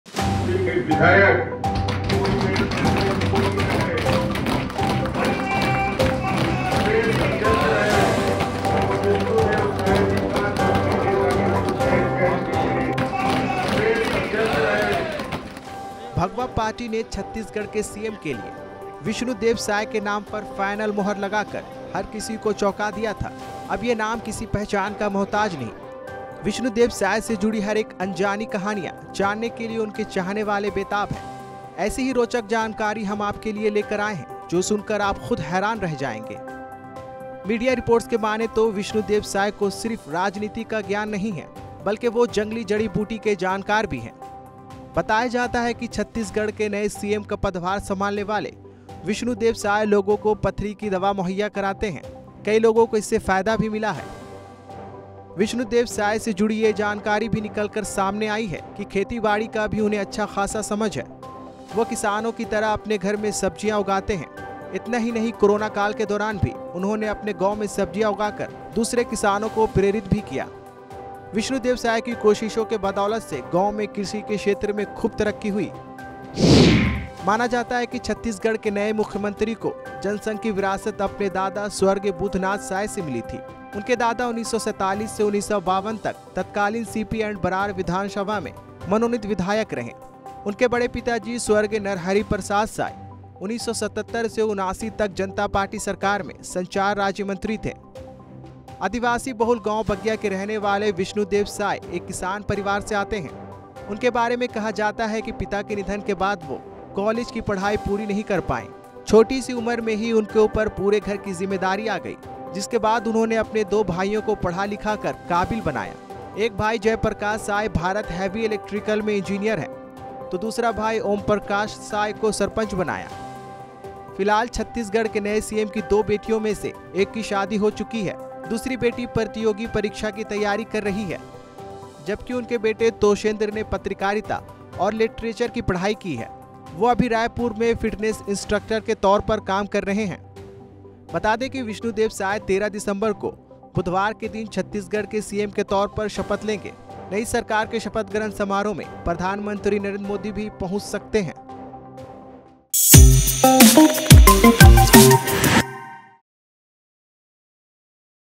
Chhattisgarh CM: Vishnu Deo Sai राजनीति के अलावा इस काम में भी हैं माहिर, देखिए | ABP Live
भगवा पार्टी ने छत्तीसगढ़ के सीएम के लिए विष्णु देव साय के नाम पर फाइनल मुहर लगाकर हर किसी को चौंका दिया था.. अब ये नाम किसी पहचान का मोहताज नहीं..विष्णु देव साय से जुड़ी हर एक अनजानी कहानियां जानने के लिए उनके चाहने वाले बेताब हैं..ऐसी ही रोचक जानकारी हम आपके लिए लेकर आए हैं..जो सुनकर आप खुद हैरान रह जाएंगे…